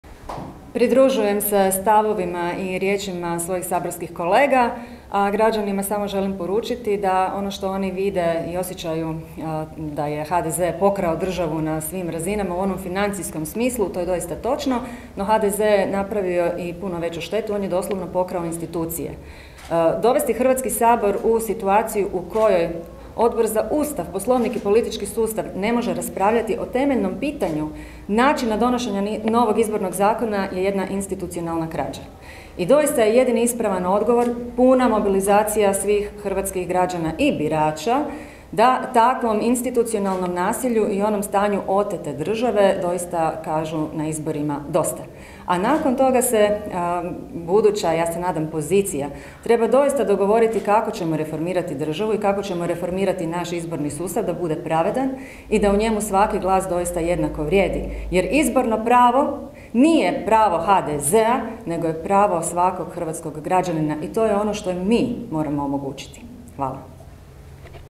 Orešković: "Izborno pravo nije pravo HDZ-a već svakog hrvatskog građana"
Na konferenciji za medije održanoj u Saboru o današnjoj sjednici Odbora za Ustav govorila je Dalija Orešković.